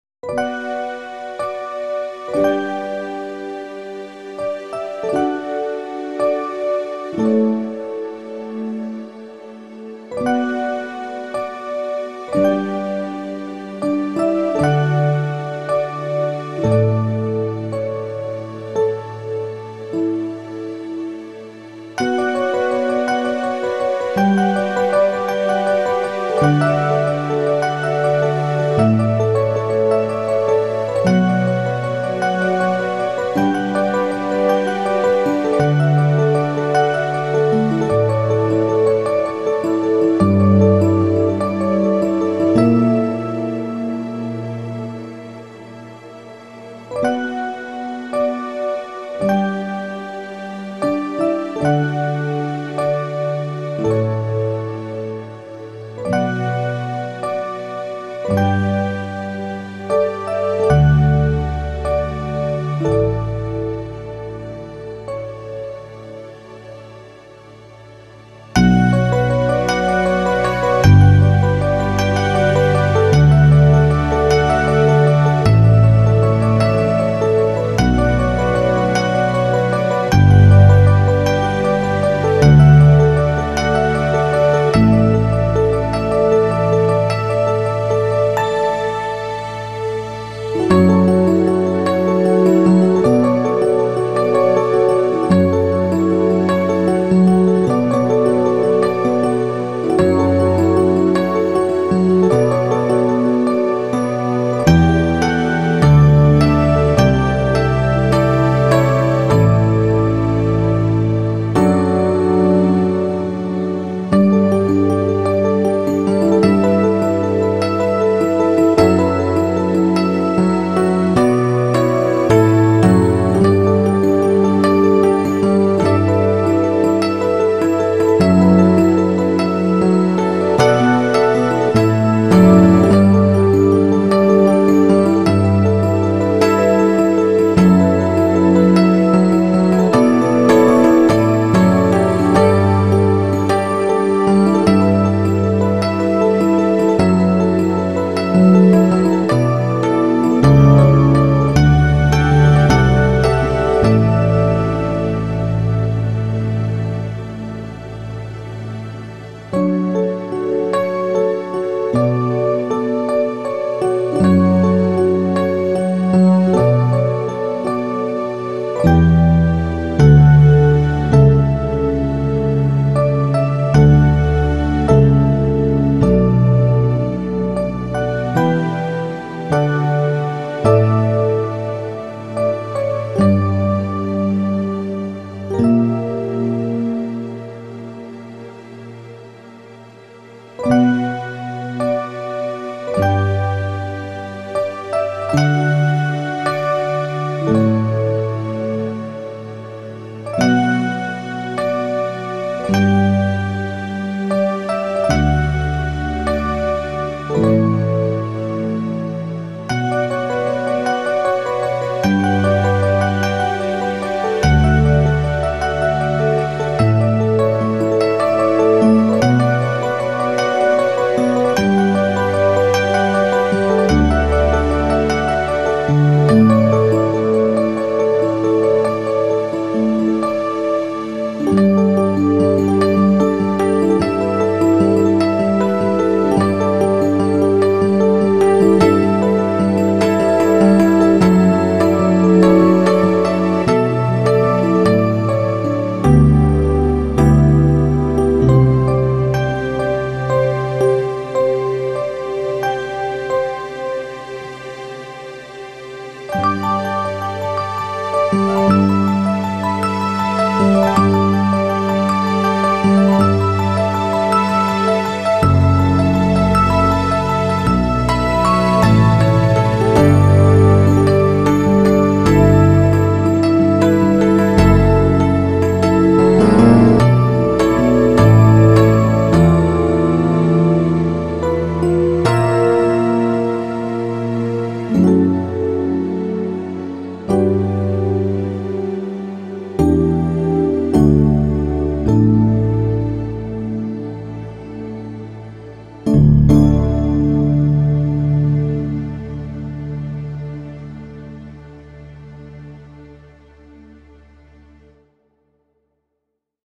Improviso Dominical (14/05/2023)
Apreciando os timbres do clássico KORG TRITON, alguns reverbs intencionais, umas ideias e deixa fluir... 😌